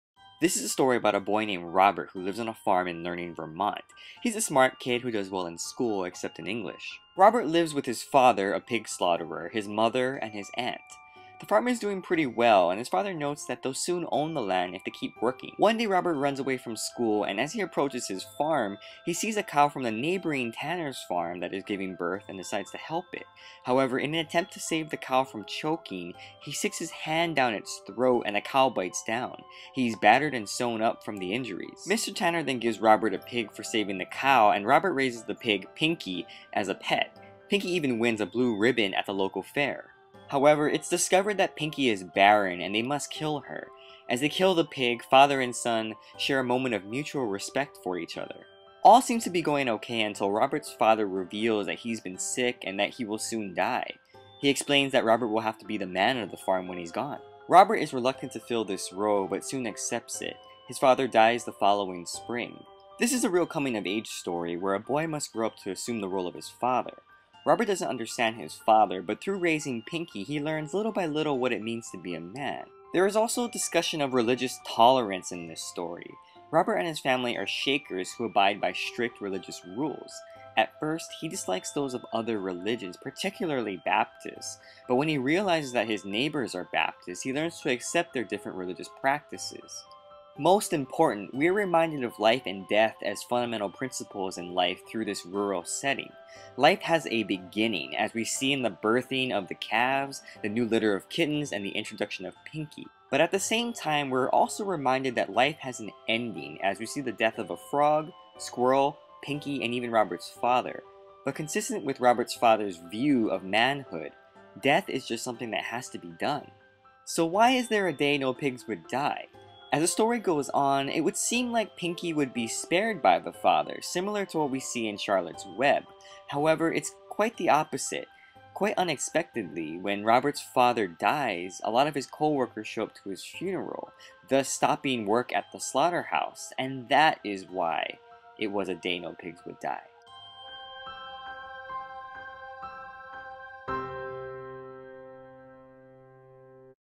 A Day No Pigs Would Die by Robert Newton Peck (Book Summary and Review) - Minute Book Report
This is a quick book summary and analysis of A Day No Pigs Would Die by Robert Newton Peck.  This channel discusses and reviews books, novels, and short stories through drawing...poorly.

This is a story about a boy named Robert who lives on a farm in Learning, Vermont.  He is a smart kid who does well in school, except in English.

Robert lives at home with his father, a pig slaughterer, his mother and his aunt.  The farm is doing well and his father notes that they'll soon own the land if they keep working.

One day Robert runs away from school and as he approaches his farm, he sees a cow from the neighboring Tanner's farm that is giving birth and helps it.  However, in an attempt to save the cow from choking, he sticks his hand down it's throat and the cow bites down.  He is battered and sewn up from the injuries.

Mr. Tanner then gives Robert a pig for saving the cow and Robert raises the pig, Pinky, as a pet.  Pinky even wins a blue ribbon at the local fair.

However, it is discovered that Pinky is barren and they must kill her.  As they kill the pig, father and son share a moment of mutual respect for each other.

All seems to be going okay until Robert's father reveals that he's been sick and that he will die soon.  He explains that Robert will have to be the man of the farm.

Robert is reluctant to fill this role, but soon accepts it.  His father dies the following spring.

This is a real coming-of-age story where a boy must grow up to assume the role of his father.  Robert doesn't understand his father, but through raising Pinky, he learns little by little what it means to be a man.  Ultimately, a man is defined by someone who does what needs to be done no matter what.

Readers see Robert assume this role after his father's death in the last chapter when he prepares the funeral arrangements and begins making business deals with his peers.

There is also discussion of religion tolerance in this story.  Robert and his family are Shakers who abide by strict religious rules.  At first, he dislikes those of other religions, particularly Baptists.  But when he realizes that his neighbors are Baptists, he learns to accept their different religious practices.

Lastly, we are reminded of life and death as fundamental principles in life through this rural setting.  Life has a beginning, as we see in the birthing of the calves, the new litter of kittens, and the introduction of Pinky.

At the same time, we are also reminded that life has an ending, as we see the death of a frog, squirrel, Pinky, and Robert's father.  But consistent with Robert's father view of manhood, death is just something that has to be done.

So why is there a day no pigs would die?  As the story goes on, it would seem like Pinky would be spared by the father, similar to what we see in Charlotte's Web.  However, it's quite the opposite.  Quite unexpectedly, when Robert's father dies, a lot of his co-workers show up to his funeral, thus stopping work at the slaughterhouse.  And that is why it was a day no pigs would die.